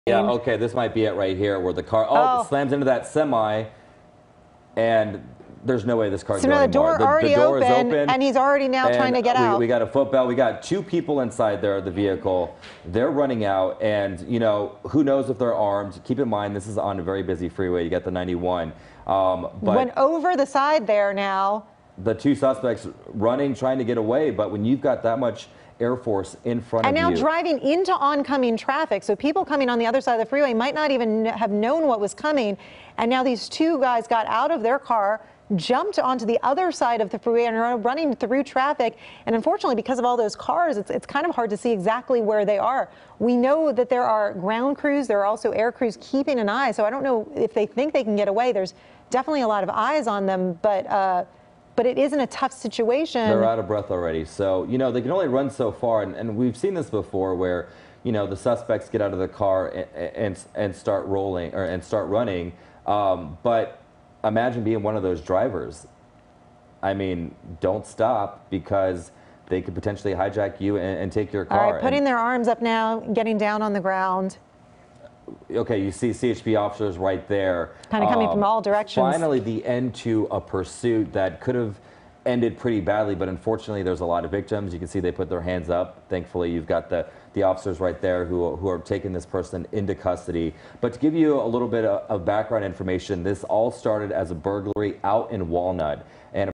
2 suspects finally surrender after wild pursuit
The chase ended with a major crash into a big rig truck.